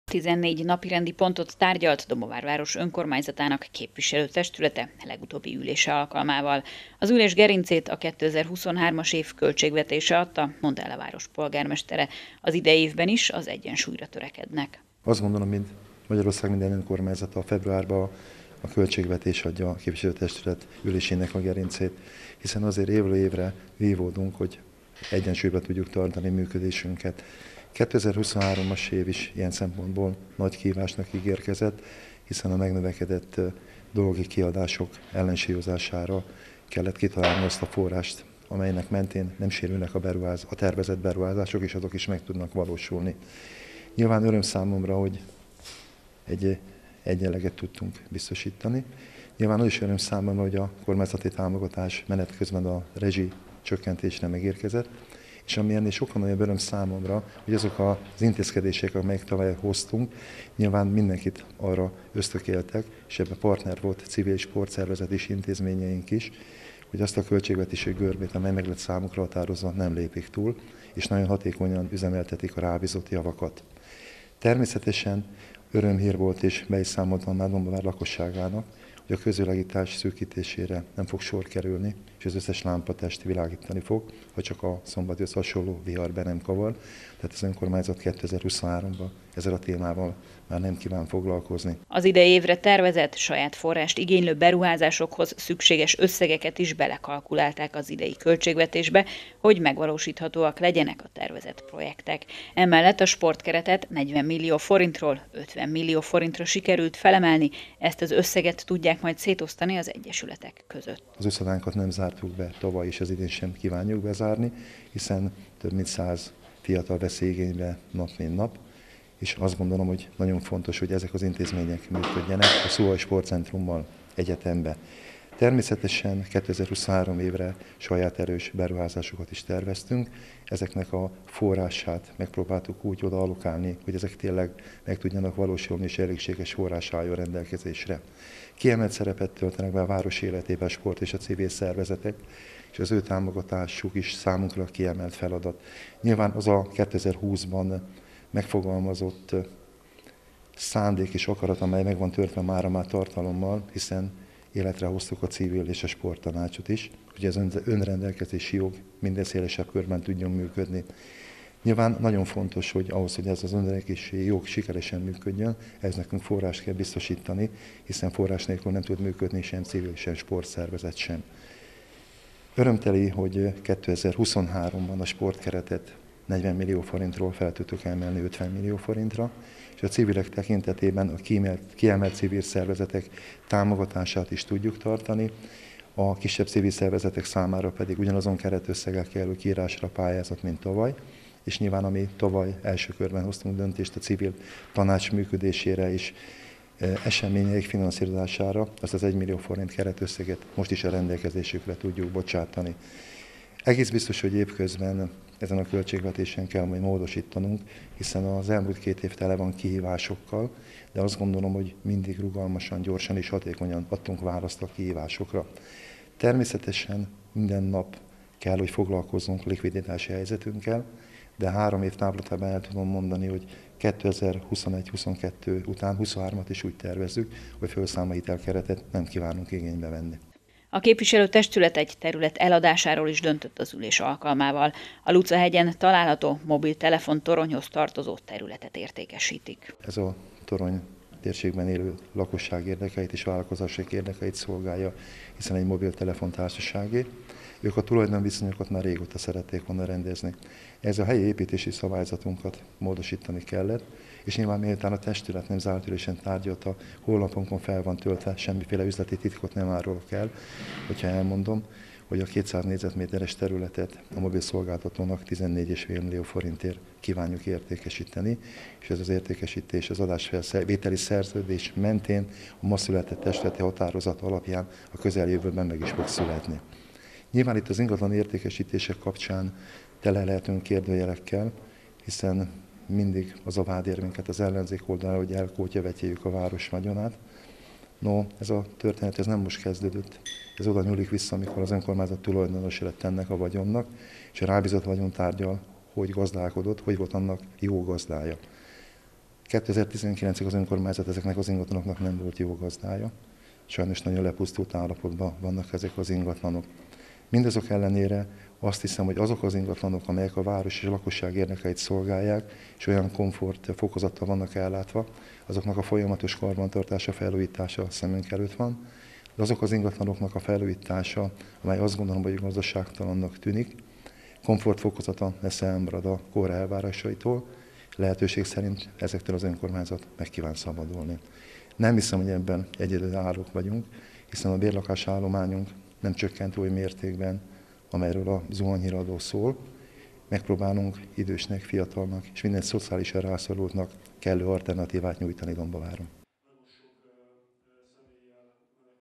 Dombóváron 40 millió forintról 50 millió forintra emelték a sportkeretet
Kaposmenti magazin - testületi ülés

A 2023-as év nagy kihívásnak ígérkezik - fogalmazta meg Dombóvár város polgármestere, Pintér Szilárd. A megnövekedett dologi kiadások finnanszírozása okozza a legnagyobb fejtörést számukra, akárcsak a legtöbb településnek.